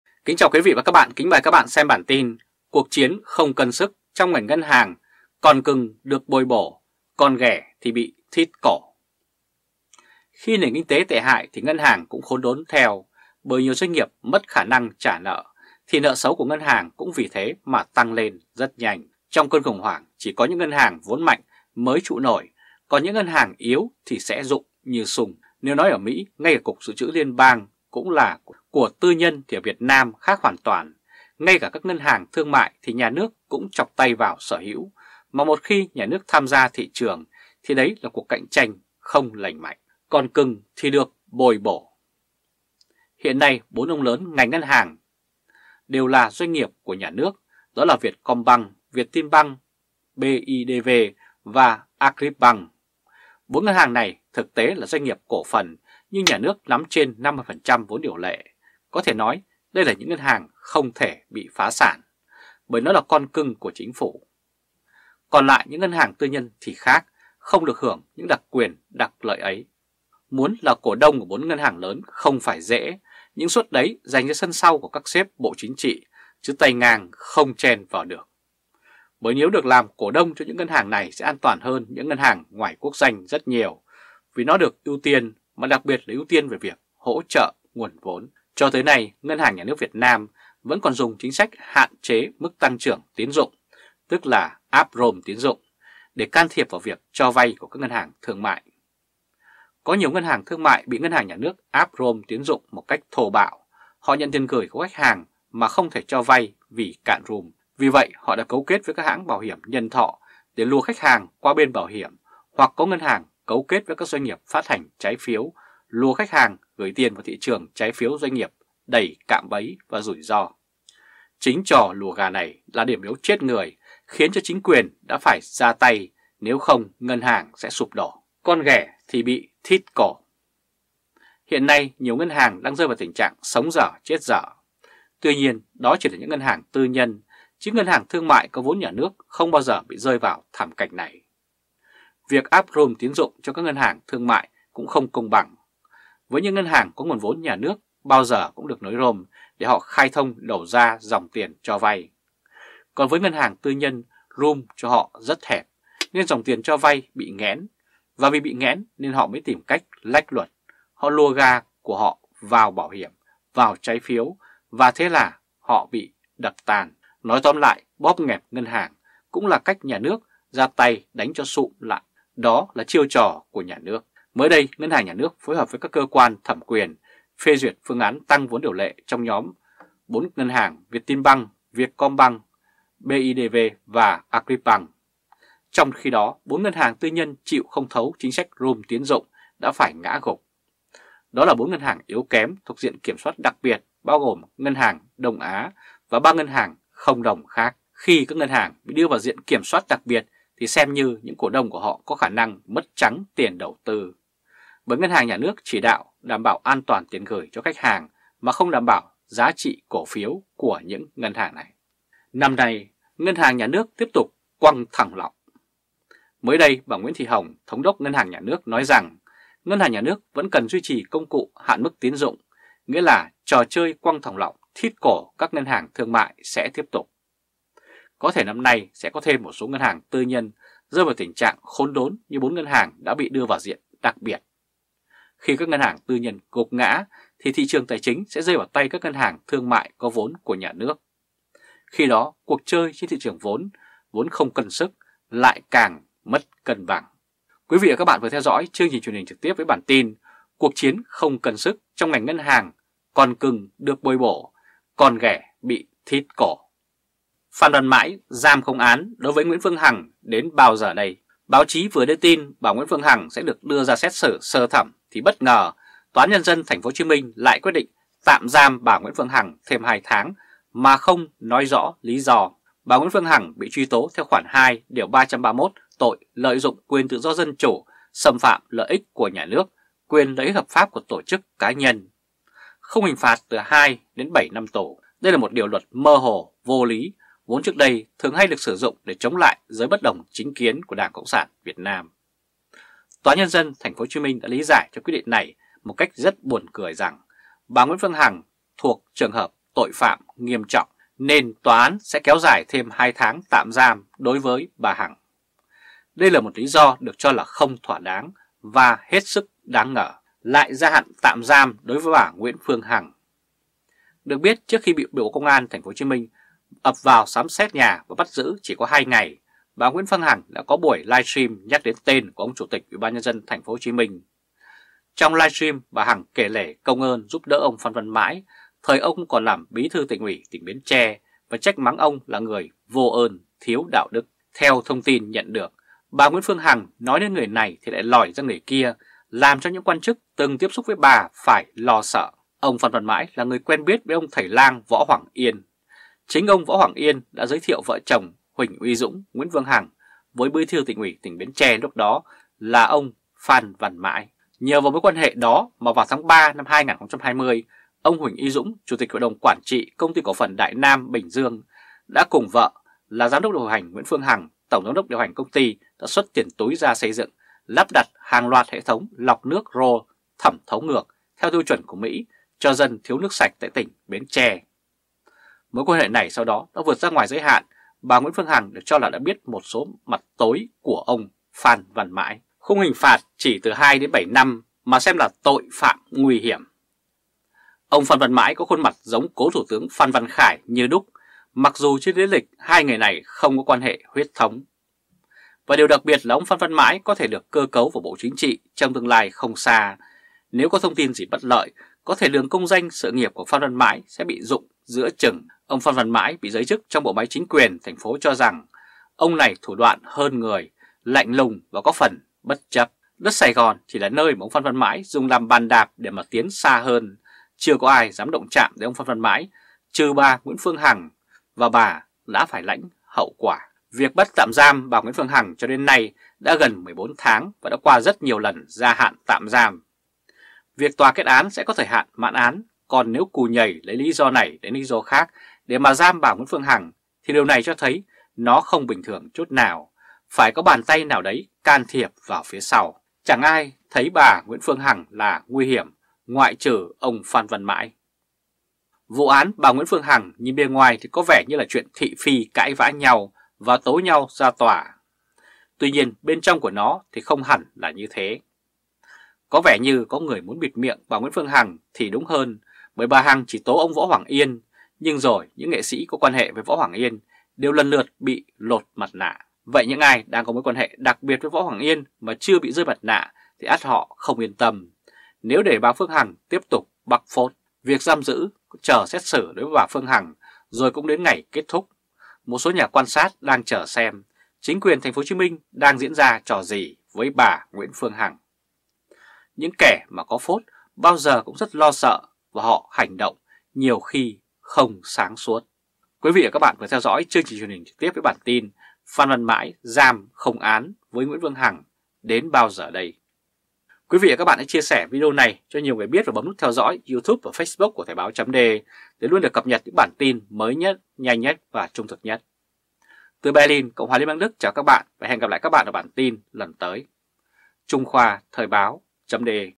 Cuộc chiến không cân sức trong ngành ngân hàng, con cưng được bồi bổ, con ghẻ bị “thít cổ”
Mới đây, bà Nguyễn Thị Hồng, Thống đốc Ngân hàng Nhà nước, nói rằng, Ngân hàng Nhà nước vẫn cần duy trì công cụ hạn mức tín dụng. Nghĩa là trò chơi quăng thòng lọng thít cổ các ngân hàng thương mại sẽ tiếp tục. Có thể năm nay, sẽ thêm một số ngân hàng tư nhân rơi vào tình trạng khốn đốn như 4 ngân hàng đã bị đưa vào diện đặc biệt. Khi các ngân hàng tư nhân gục ngã, thì thị trường tài chính sẽ rơi vào tay các ngân hàng thương mại có vốn nhà nước. Khi đó, cuộc chơi trên thị trường vốn, vốn không cân sức, lại càng thêm mất cân bằng.